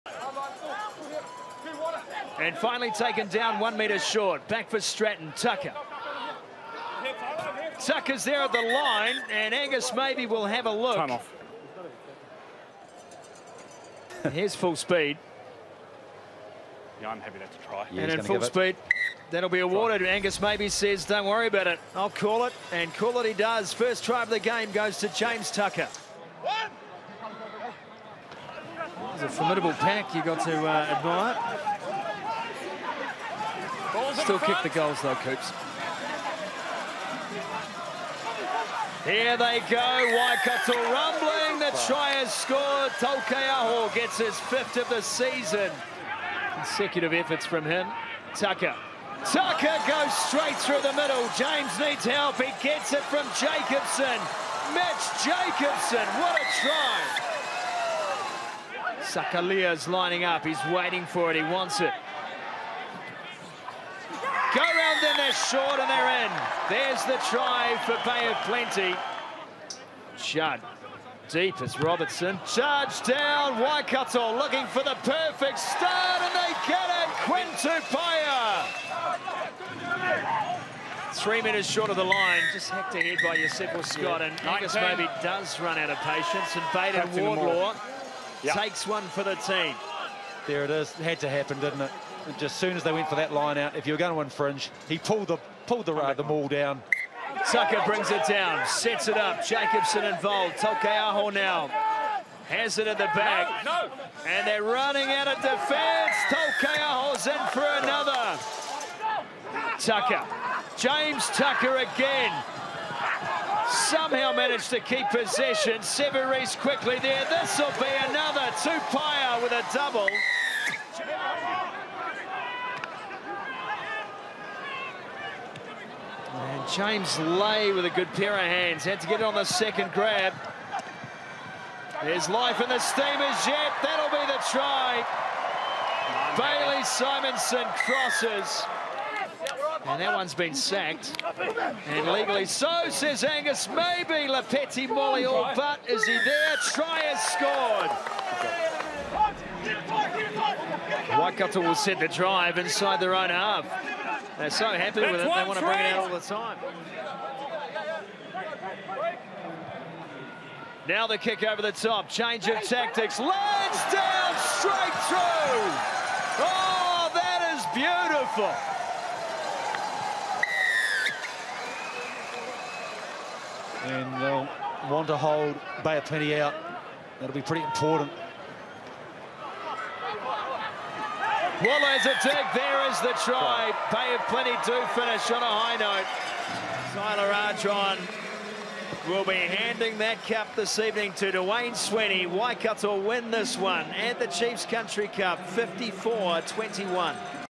ROUND 6 HIGHLIGHTS: Bay of Plenty v Waikato – 2018
Waikato have made it four straight wins and taken a stranglehold at the top of the Championship ladder with a thumping 21-54 victory over Bay of Plenty in Rotorua.